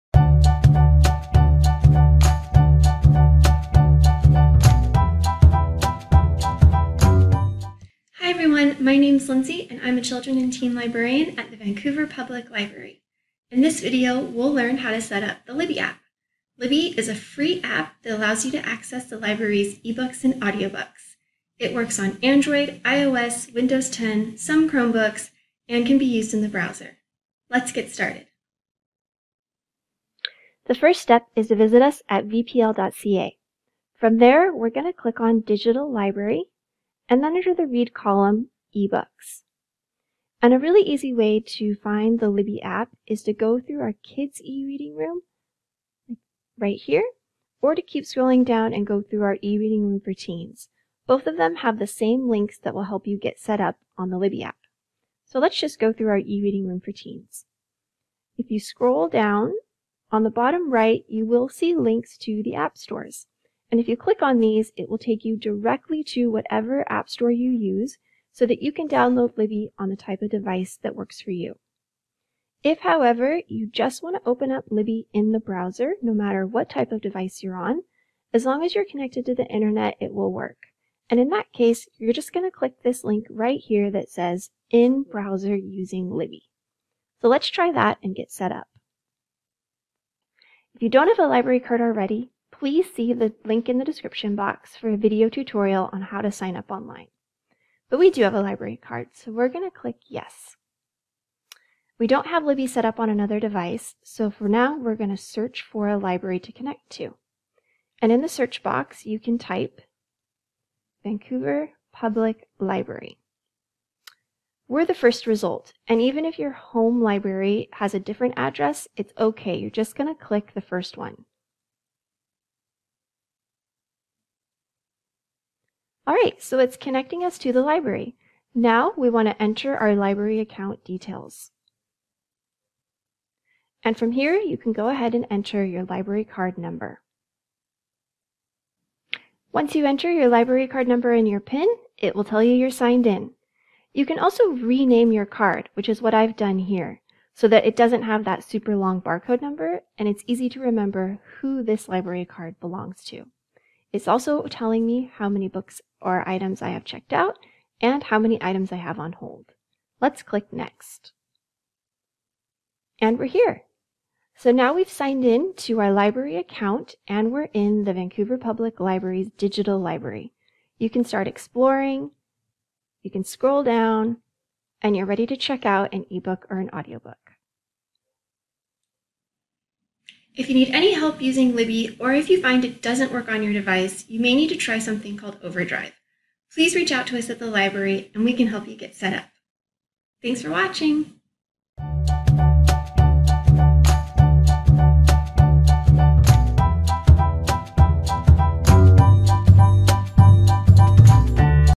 Setting Up Libby for Kids and Teens: Part 1
Learn how to set up your account and connect to the library using the Libby app on a device or in your browser.

Questions?